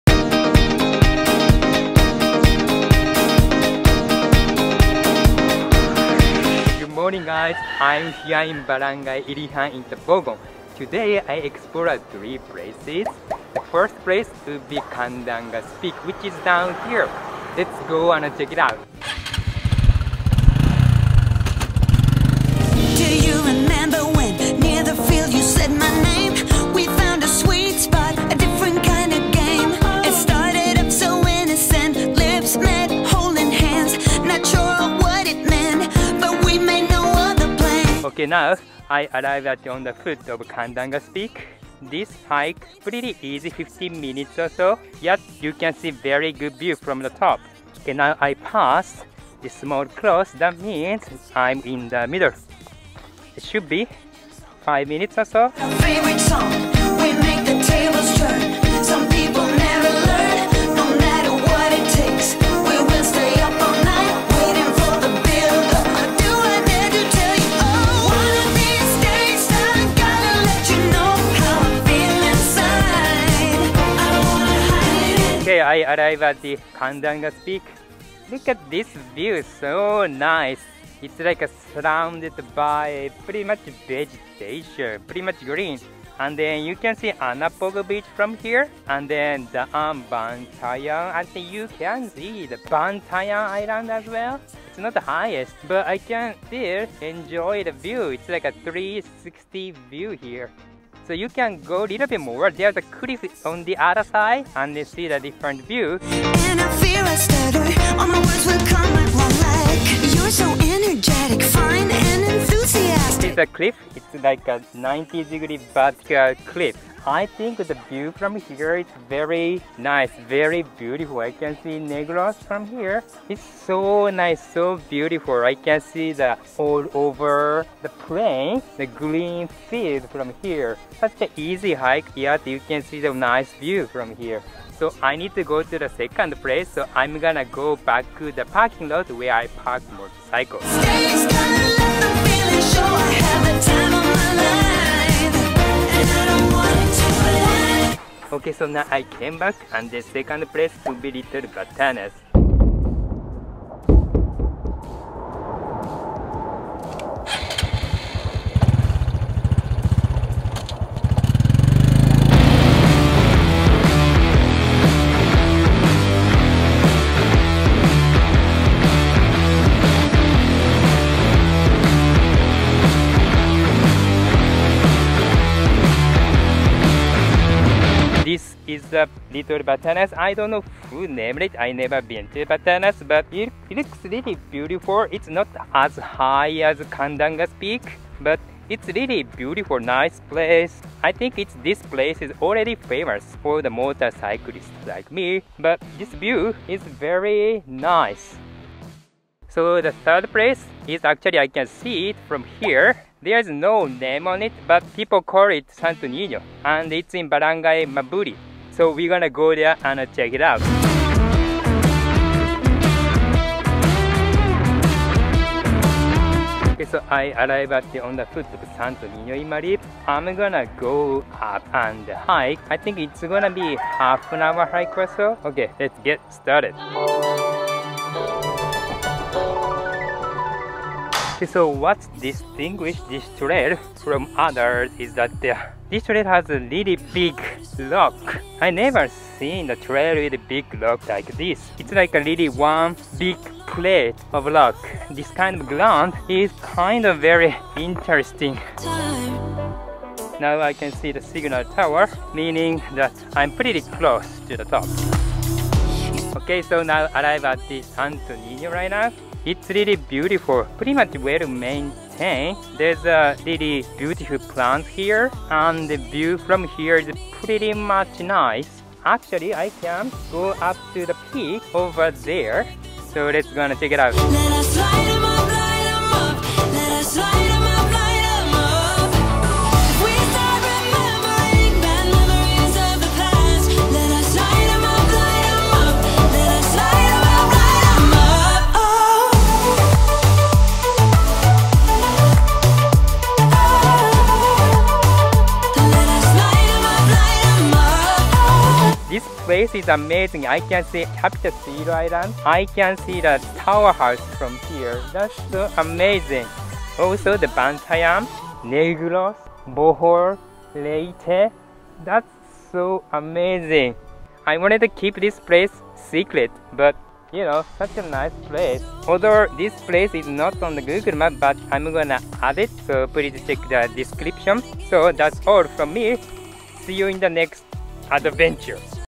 I Found A HIDDEN Place in Cebu Philippines 🇵🇭 | Tabogon
Tabogon is located in the North Province of Cebu and offers several famous tourist spots alongside its coast such as beaches and spring. However, Tebogon has good hiking trails in its mountainous areas.

In this vlog, I explored three places near Central Nautical Highway.

My first stop was Candangas Peak, which is a pretty easy hike to reach the top, but you can see the amazing view from there. There's a cliff on the other side, so I recommend going the extra mile to see the wonderful view.

The second stop was Little Batanes, which is already popular, so I think that I don't need to explain so much about it.

The third stop is tricky because it's a bit hard to get there. I think that the Sto. Niño is very less known to the people outside the Barangay Mabuli, so it's nowhere near as touristy as the beachside activities in Tabogon, but I feel that it could potentially be one of the best tourist spots in North Province of Cebu because you can see Capitancilllo Island and the lighthouse from the top 😊😊

⏳ Timestamps

00:00 - Intro
00:18 - Going to Candangas Peak
01:03 - Candangas Peak
01:44 - Cliff in Candangas Peak
02:32 - Little Batanes
03:30 - Going to Sto. Niño in Mabuli
04:28 - Going to the Hidden Peak

📍 Landmarks

🏞️ Route I took